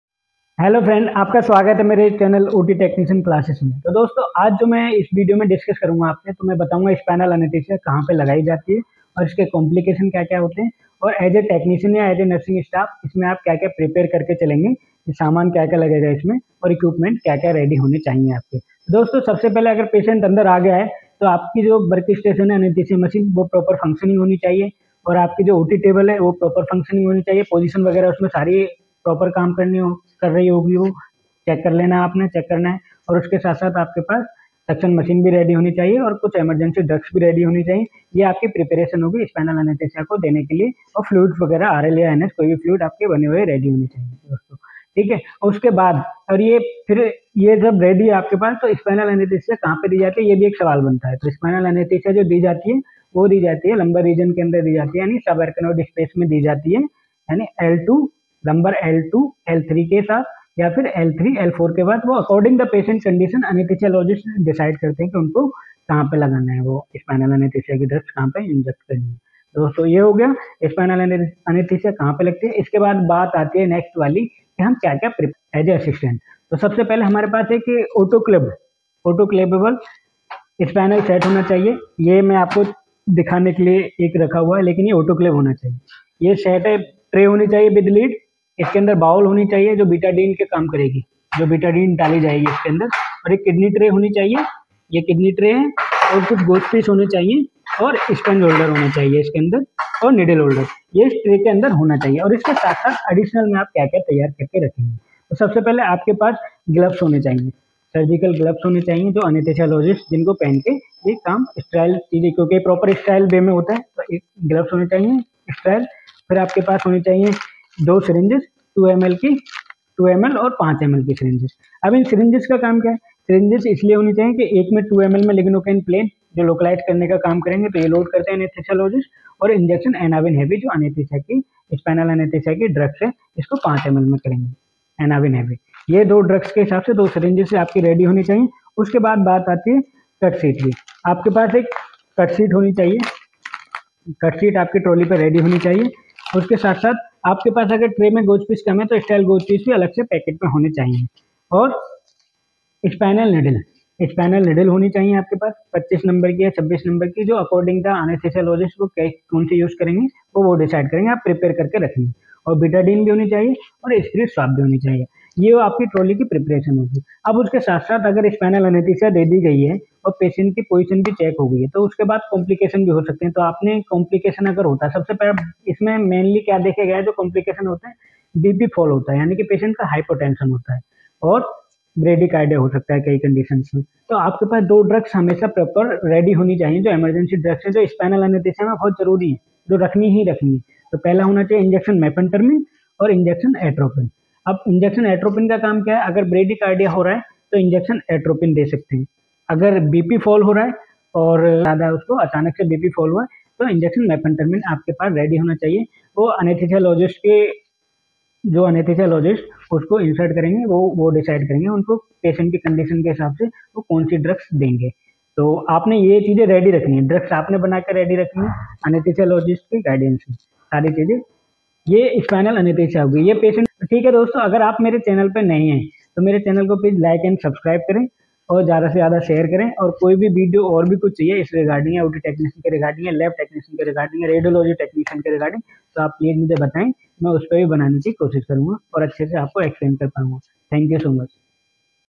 Spinal Anaesthesia trolley preparation || Spinal Anaesthesia के लिए कोन - कोन से सामान चाहिए??
Spinal anaesthesia trolley preparation. drugs name, equipment, proper explain Hindi me.